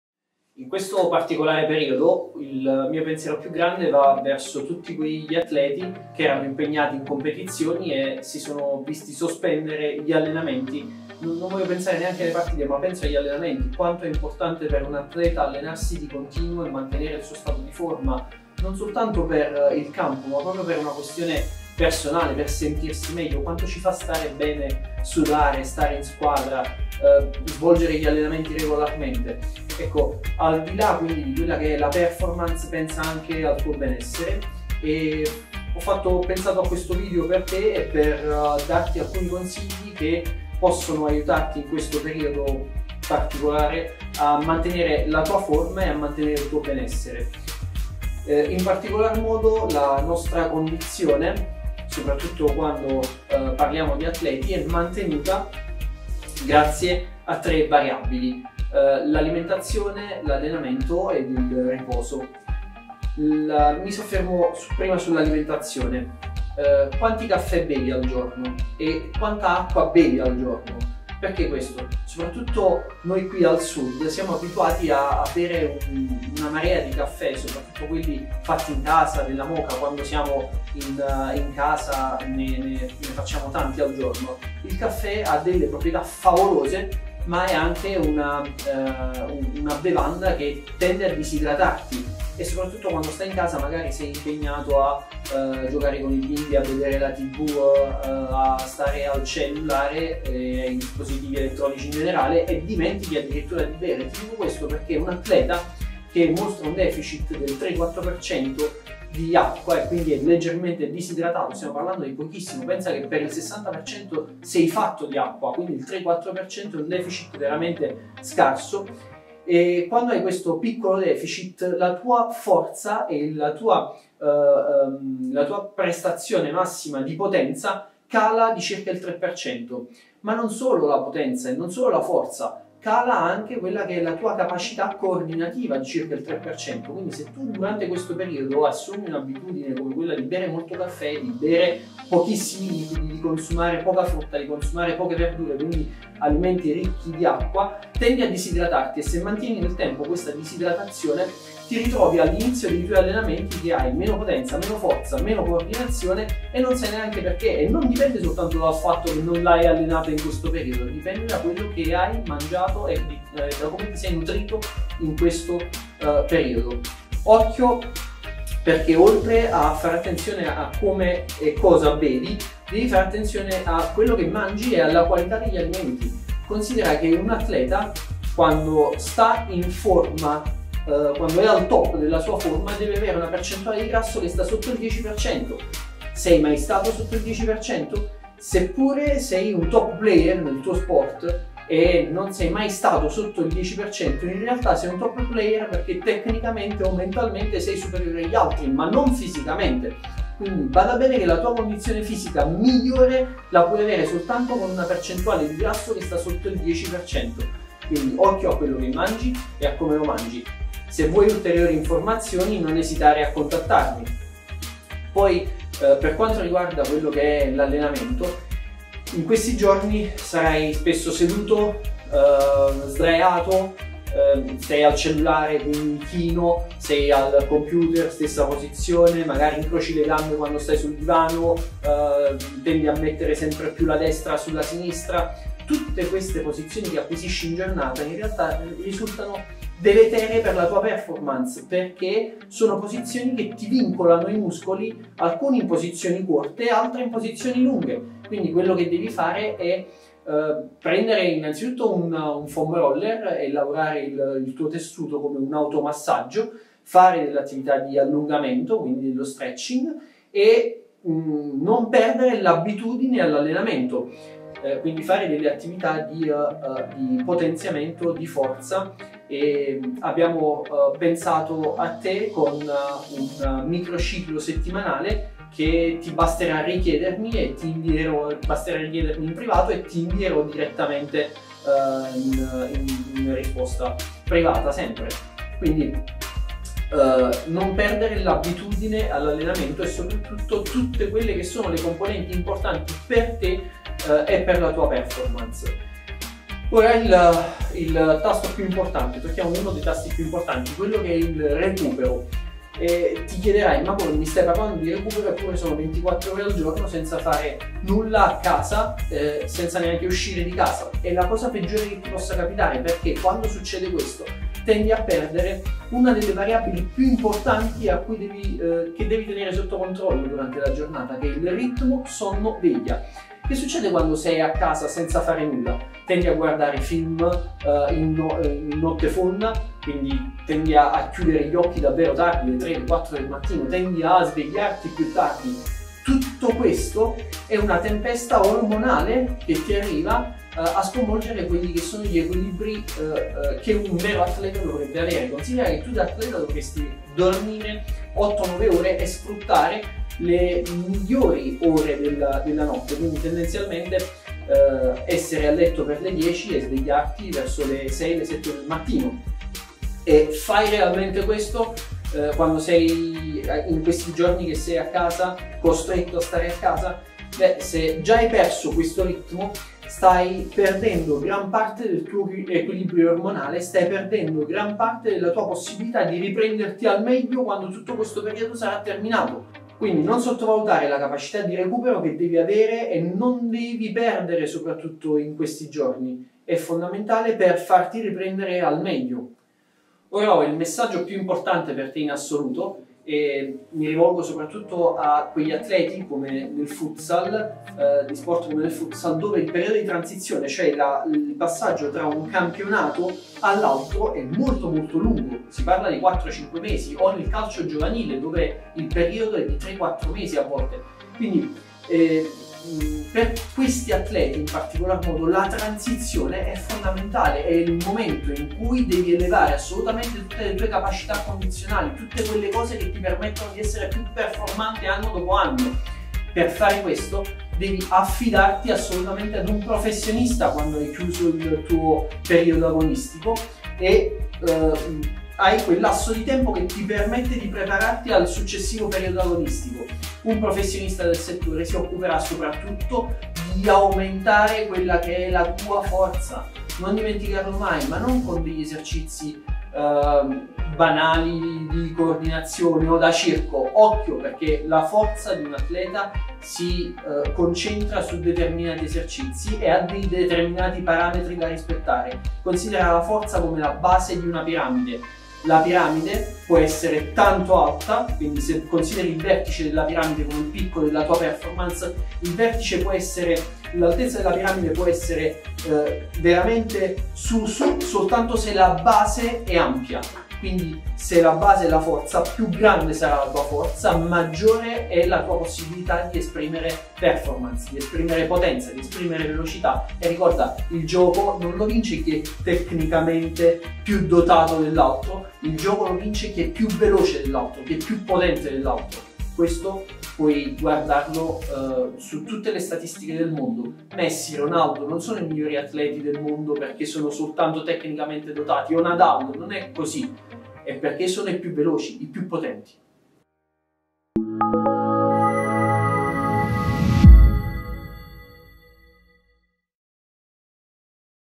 Covid-19: STAGIONE ATLETICA IN CRISI!
Come recuperare le energie e non buttare all'aria anche il prossimo anno